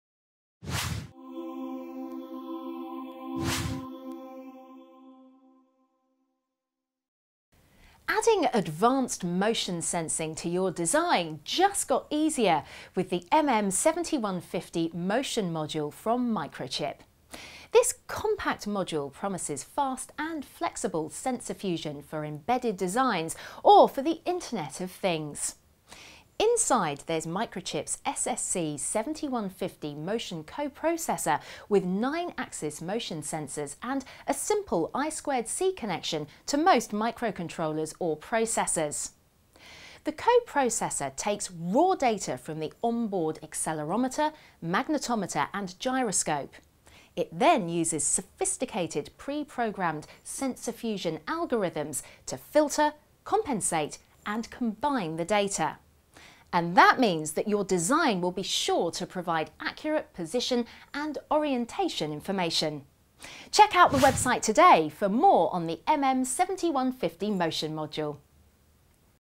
Compact Motion Module Simplifies Sensor Fusion
[MNV235] Microchip’s New Motion Module Makes Motion Monitoring Easy.